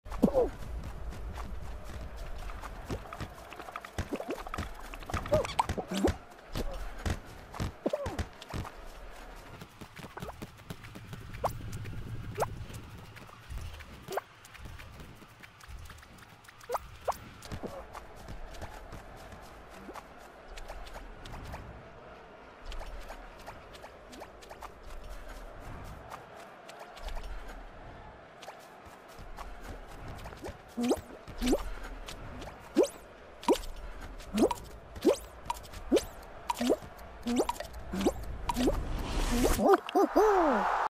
Saving Bert! [Season 6.5|Fall Ball]
In this video I saved a penguin named Bert on a map or gamemode named Fall Ball! Ignore the hastags